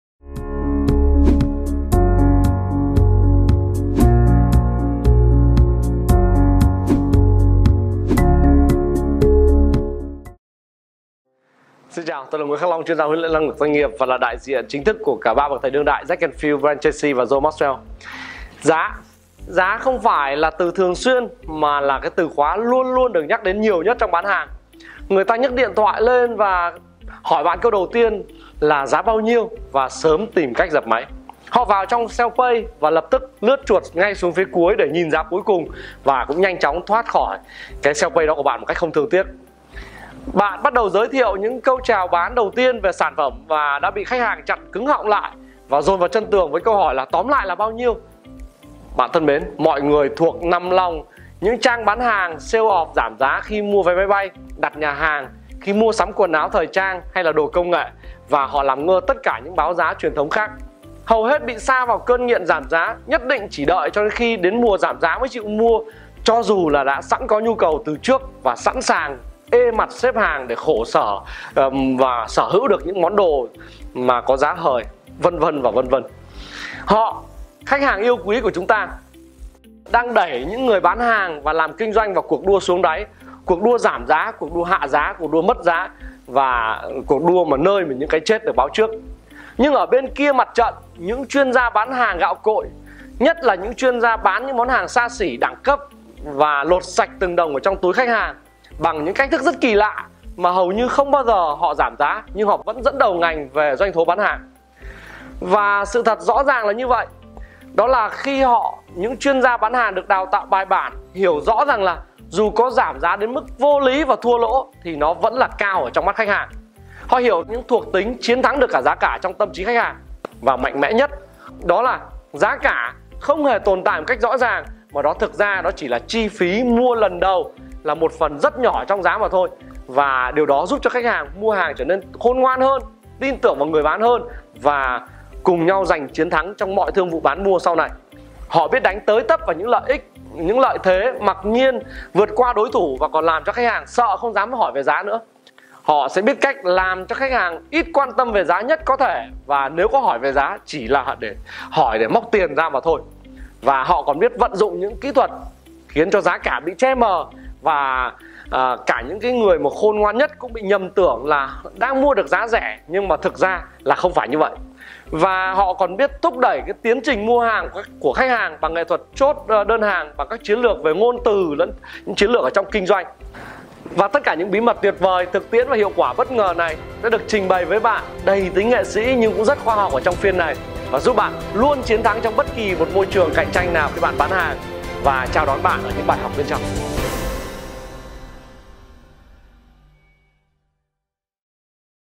Brand Camp Trailer: Price-selling: Vượt qua trở ngại về Giá (OlymWorld)
Điều khoản giá cả chắc chắn đã khiến nhiều người bán “đổ mồ hôi, sôi nước mắt” khi thương lượng với khách hàng. Tuy nhiên nếu người bán làm rõ được giá trị và điểm khác biệt của sản phẩm, người mua sẽ dễ dàng chấp nhận mức giá được đưa ra hơn. Khóa học này sẽ giúp học viên hiểu đúng về giá, nhận diện chính xác và xử lý hiệu quả những từ chối phổ biến về giá.